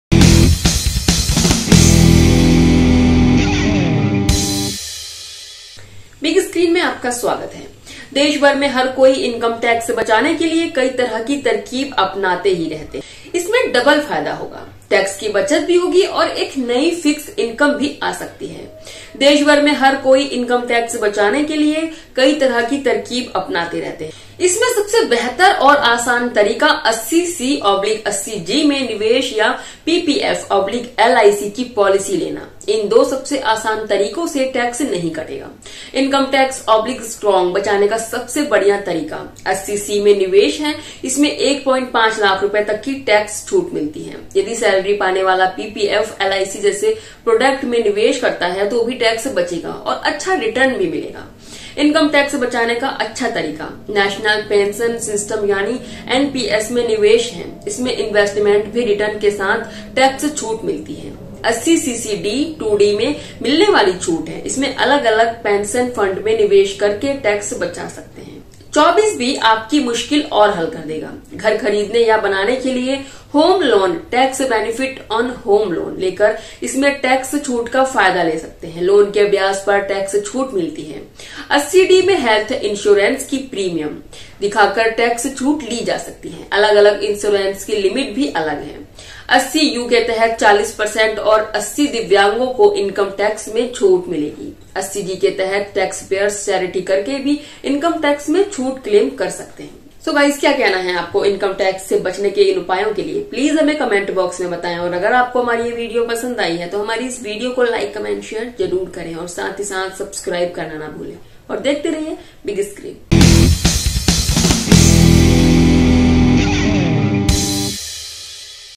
Salary से नहीं कटेगा Income Tax, 2 नियम जानने से लाखों की बचत
Everyone across the country keeps adopting various tricks to save these low taxes. Tax Consultant Advocate Jitendra Verma's exclusive conversation with Patrika.com. Which will have double benefit. There will also be tax savings and a new fix-in may also come in less. The best and easiest way in this is to invest in 80C / 80G or take a PPF / LIC policy. Tax will not be deducted in these two easiest ways.